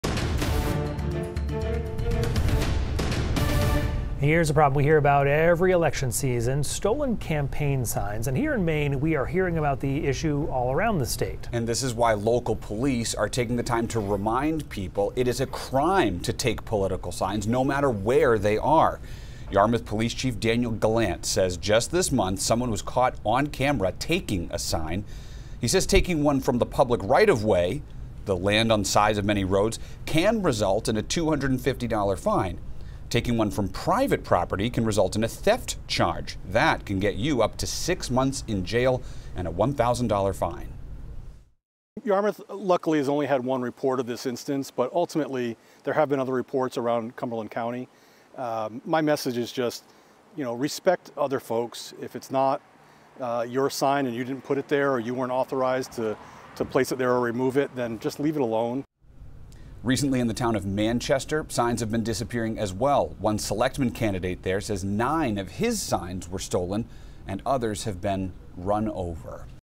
Police: It's a crime to take political signs, no matter where they are
It's a problem we hear about every election season: stolen campaign signs. Here in Maine, we're hearing about the issue all around the state.